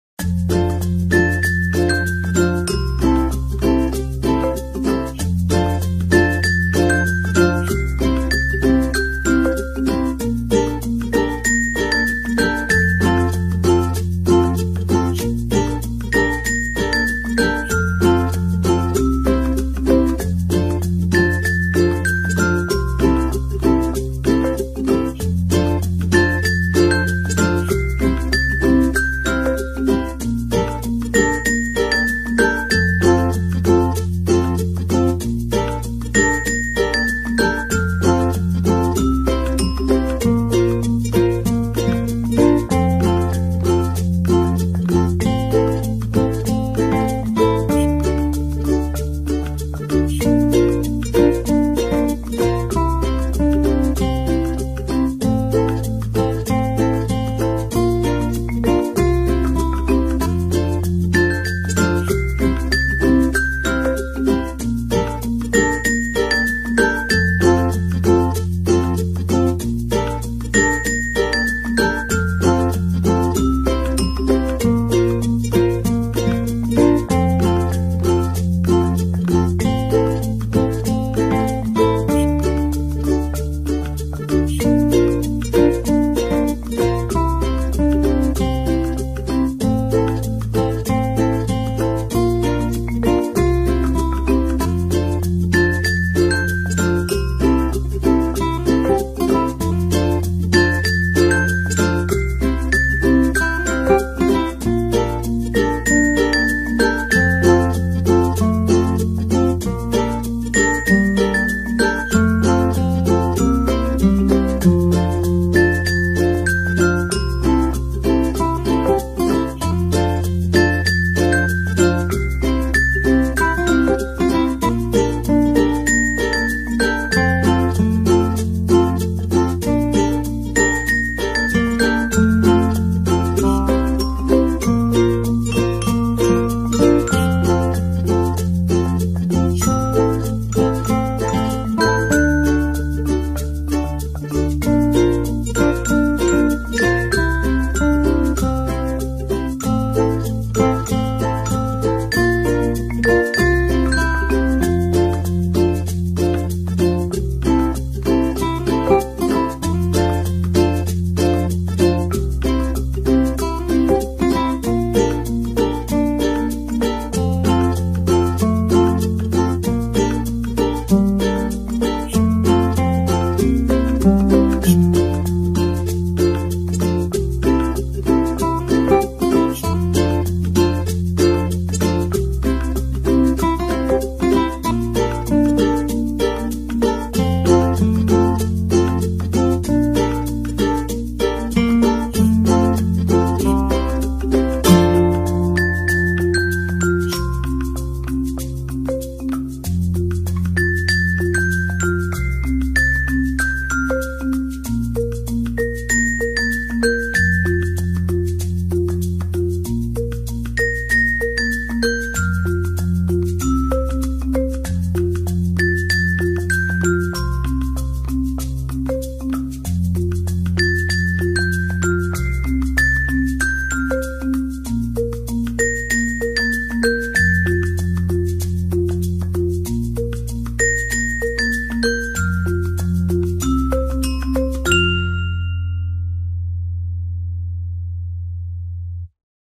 Sada patek pro overlock Leader VS340D
Patky rozšiřují možnosti šití s overlockem. Sada obsahuje řasící patku, patku pro slepý steh, patku na přišívání korálek / perliček, patku na přišívání gumičky a kedrovací patku.